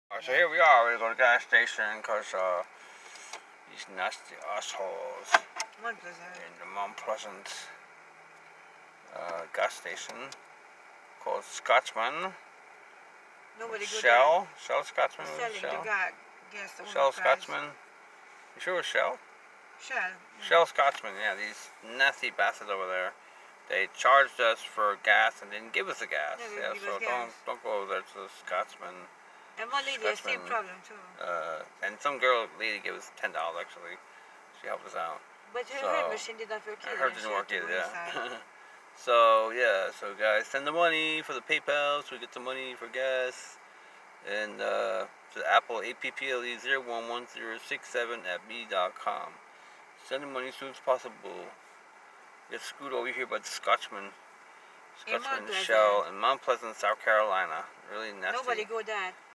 Patrick lallys Homeless American life in the USA(23)
Travel Vlog of this travelling family who was kicked out of his parents house by the Evil Sisters on 2017.   So everyone please help us.   Thanks